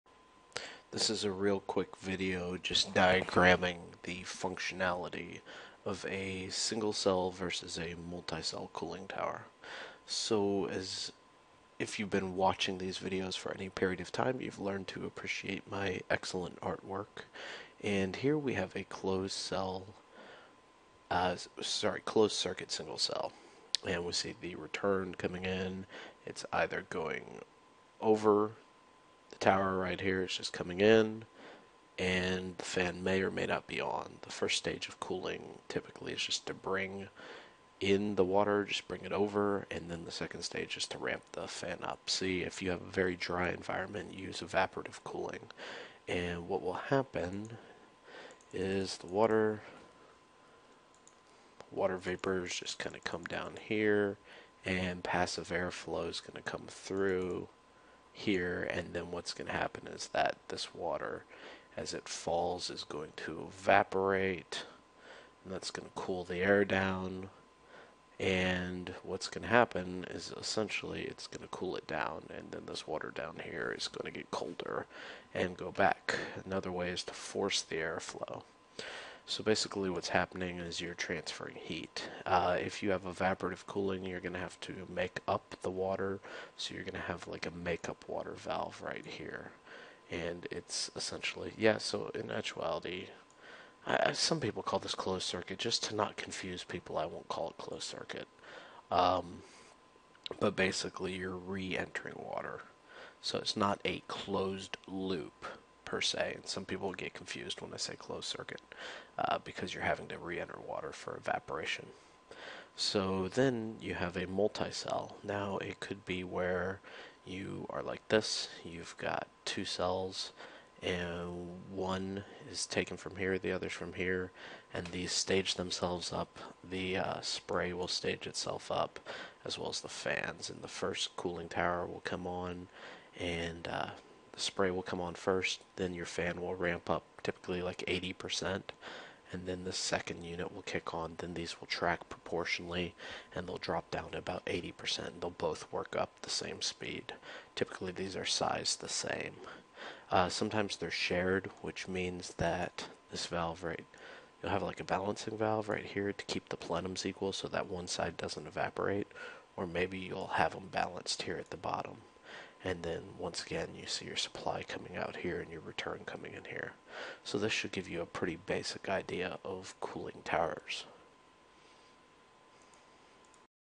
Cooling Tower Basics
In this video I briefly discribe the fundamentals of cooling tower operation.